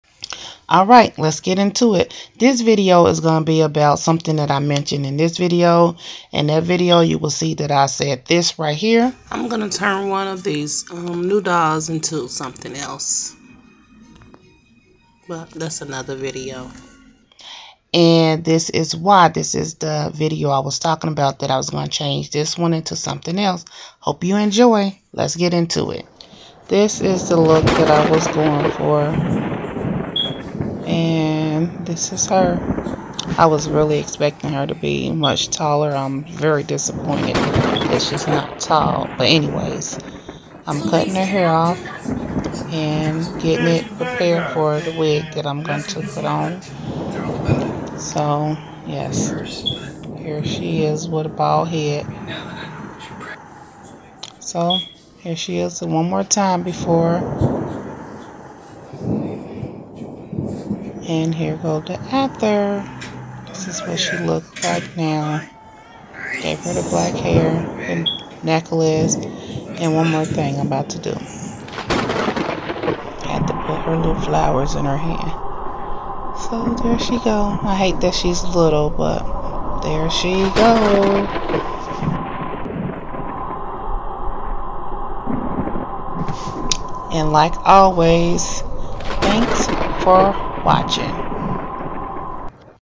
New Spencer's/Spirit Halloween Tiffany doll make over...
I turned one of my Spencer's/Spirit Halloween doll into the original doll before she turned into Tiffany. Hope you enjoy this video. you can check out my other videos on my channel.